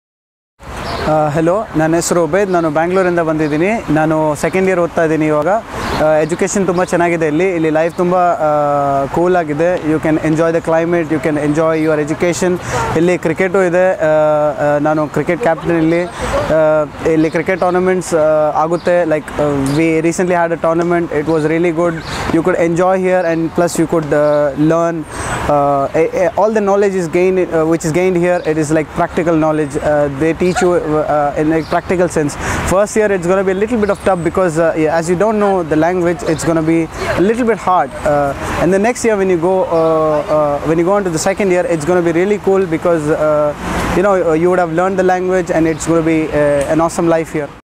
Mr. Obedulla
Mr. Obed is the current second year student at the Faculty of General Medicine English taught program. He comes from the famous city of Bangalore in southern part of India. He has been taking very active participation in sports tournament and is the captain of the juniors cricket team at our university. He has made a valuable contribution in developing the tradition of organising cricket at our university in collaboration with the team of our Indian admissions office.